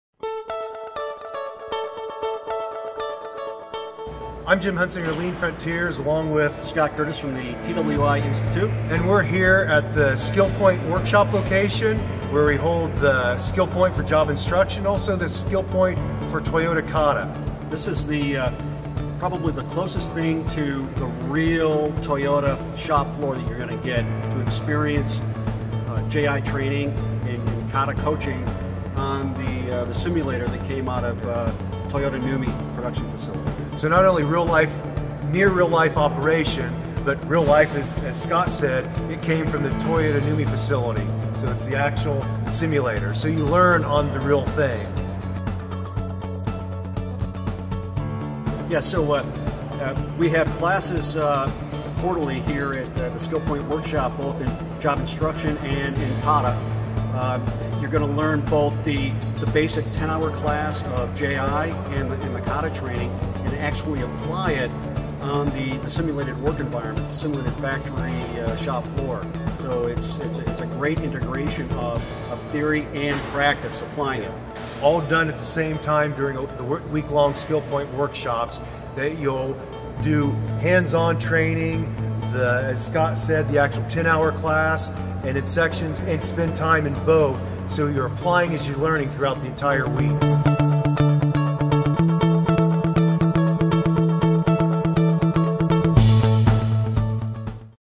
SkillPoint for Toyota Kata & TWI Job Instruction
You can LEARN Kata and TWI JI at many workshops or you can EXPERIENCE it at a SkillPoint workshop. These workshops use the very same simulation used by Toyota at the NUMMI plant to teach TPS to GM employees.